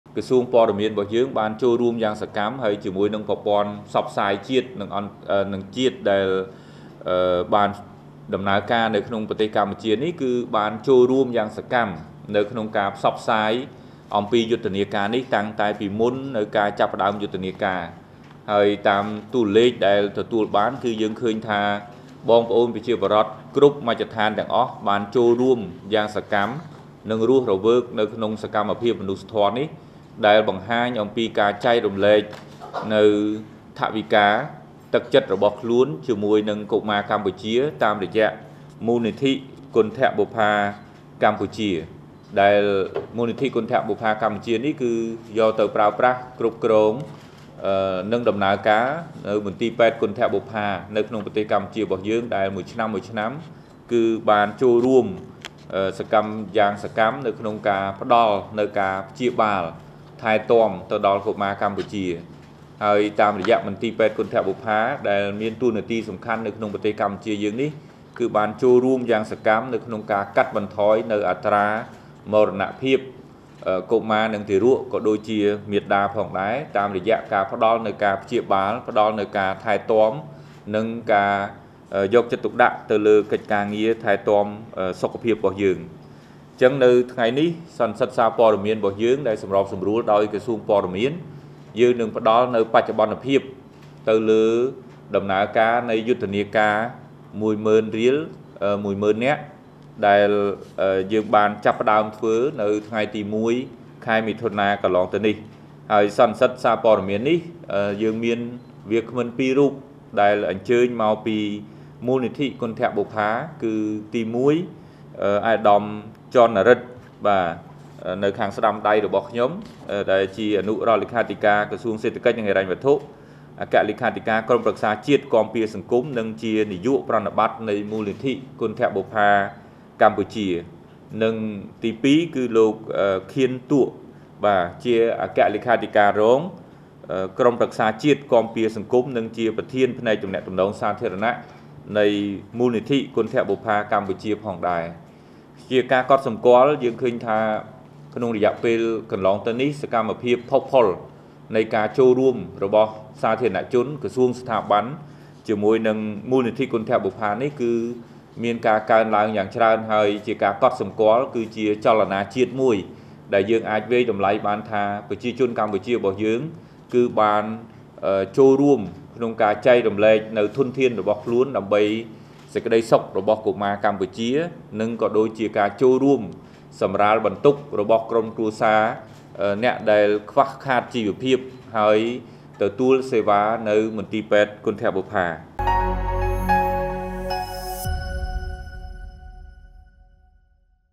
ត្រឹមរយៈពេល២២ថ្ងៃ នៃយុទ្ធនាការ “១ម៉ឺនរៀល ១ម៉ឺននាក់” មូលនិធិគន្ធបុប្ផាកម្ពុជាបានថវិកា ២,៧២ លានដុល្លារ
។ លោក នេត្រ ភក្ត្រា រដ្ឋមន្ត្រីក្រសួងព័ត៌មាន បានលើកឡើងនៅក្នុងសន្និសីទសារព័ត៌មាន នៅថ្ងៃទី១៩ មិថុនា ឆ្នាំ២០២៤ ថា តួលេខនៃការបរិច្ចាគនេះ បង្ហាញពីការចែករំលែកយ៉ាងសកម្មទាំងថវិកា និងទឹកចិត្តរបស់ប្រជាពលរដ្ឋ ដើម្បីជួយដល់កុមារកម្ពុជា។